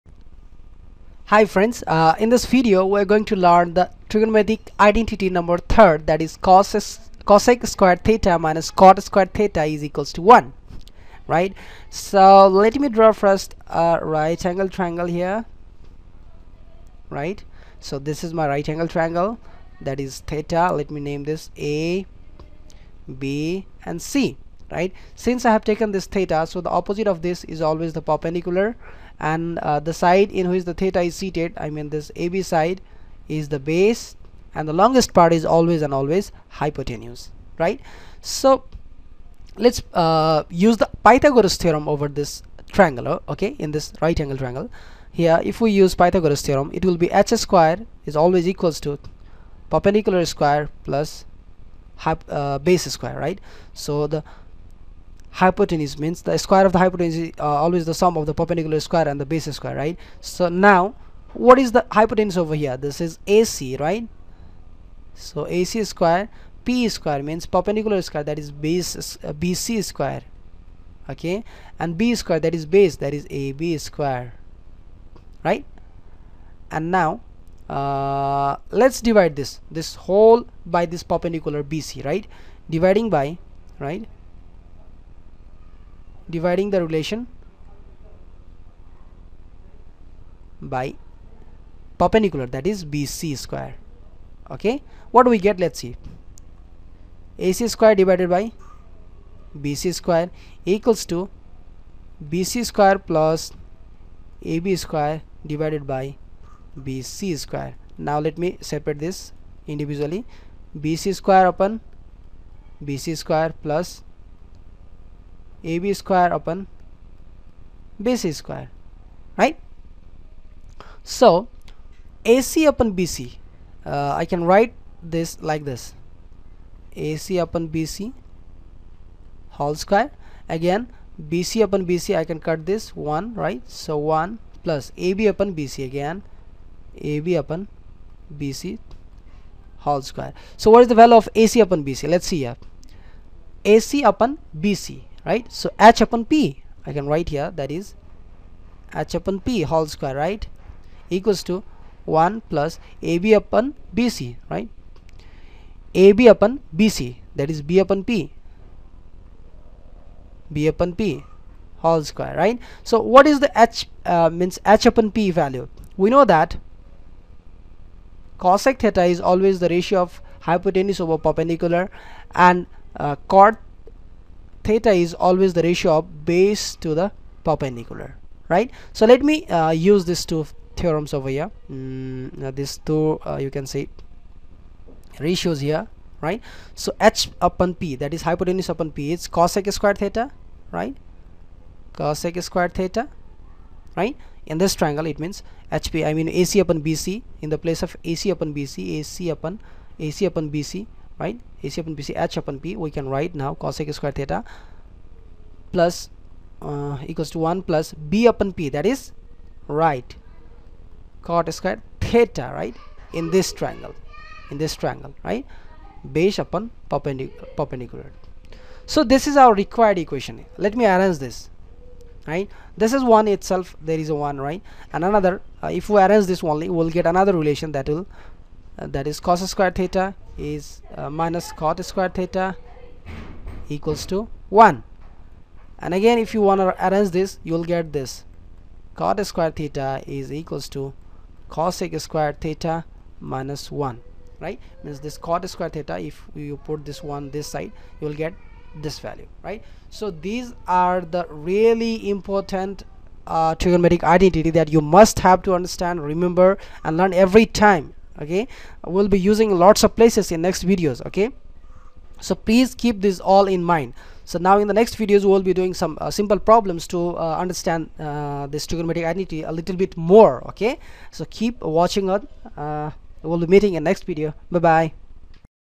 Trigonometry Identity Cosec2A - Cot 2A = 1 Proof
for complete list of free Trigonometry video lessons.
 In this video we prove the third (3rd) trigonometric identity that is Cosec2A - Cot 2A = 1. This is a very good identity and we use this trigonometric identity in lots of places.